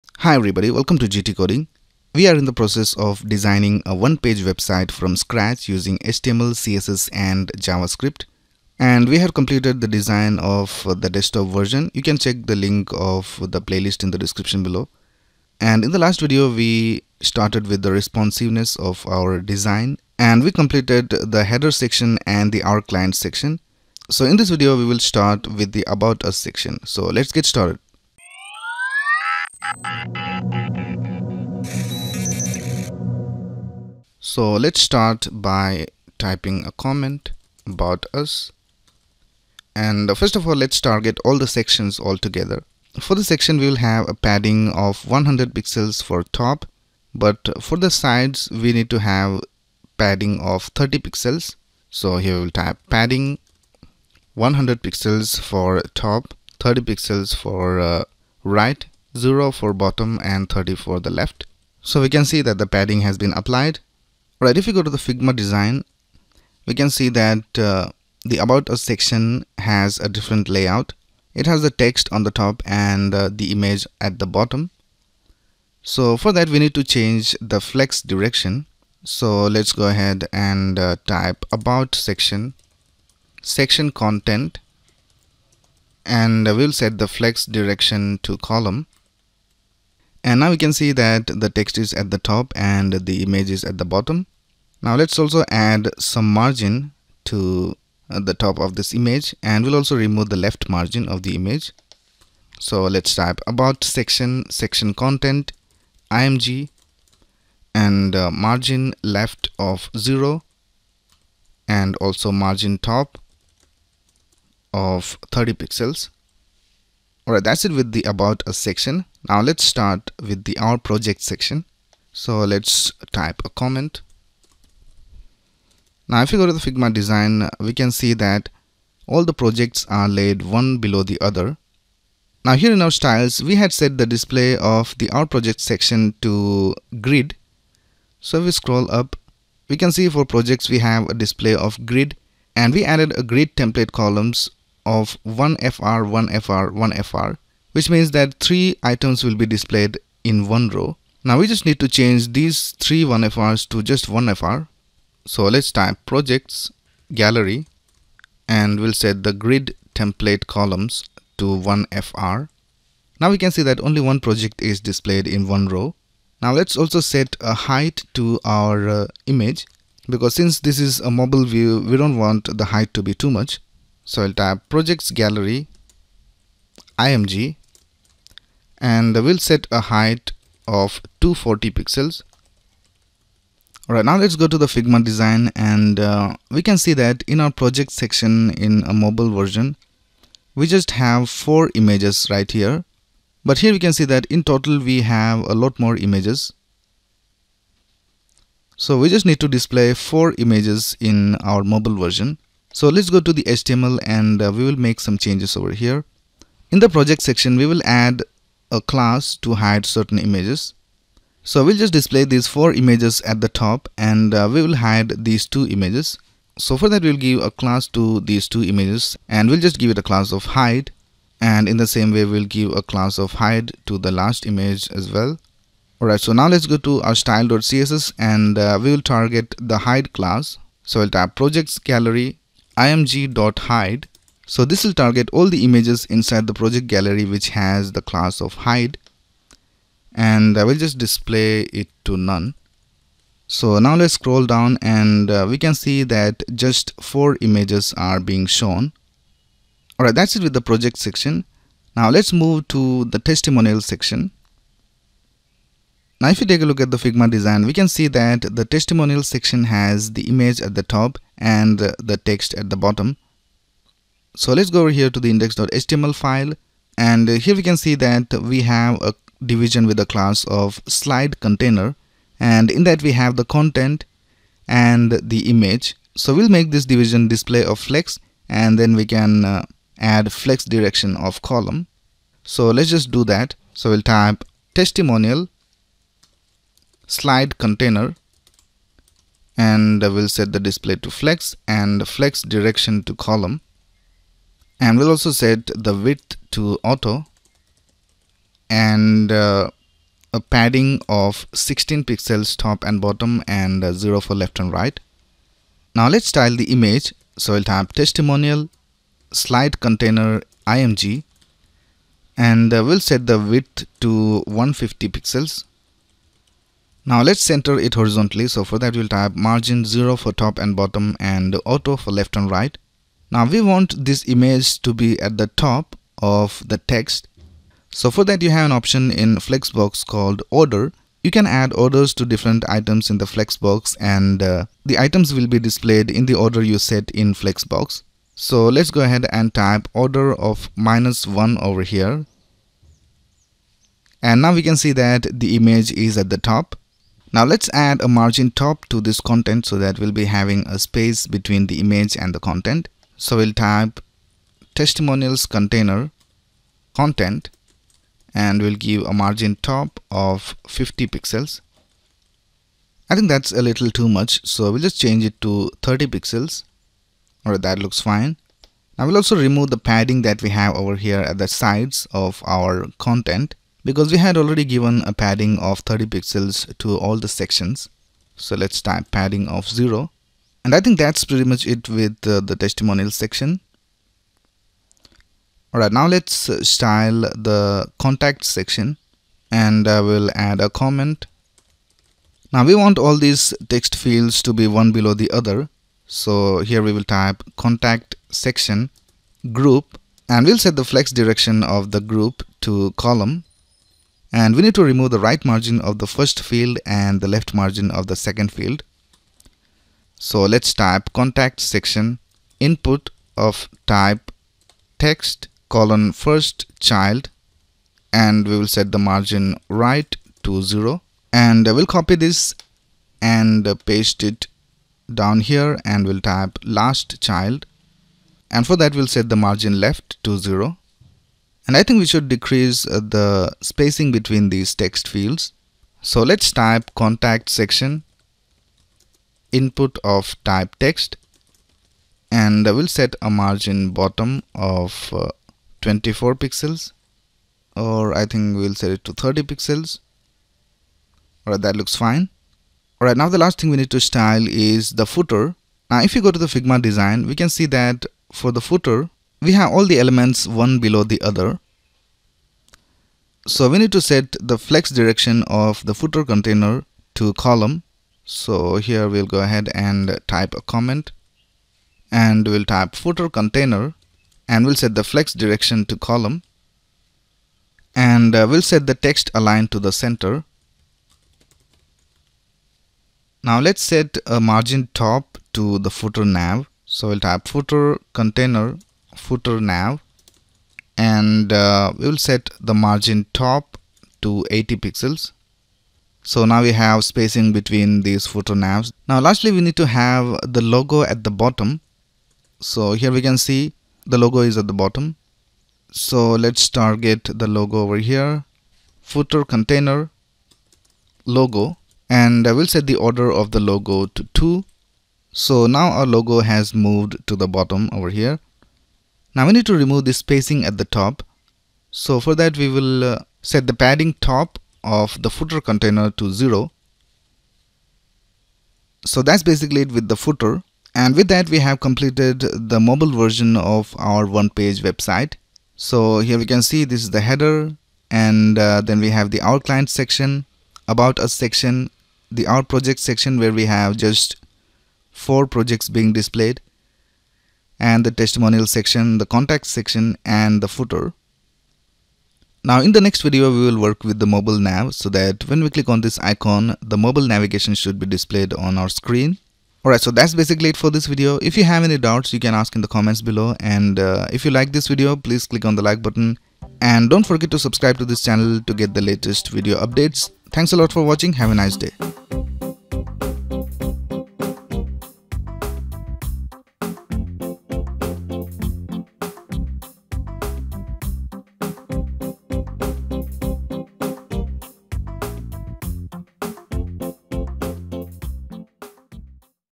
Figma To HTML - One Page Website - Responsive Design - II
Hi guys, in this video, we will design the mobile version of the rest of the sections. We had started with the responsive design in the previous video.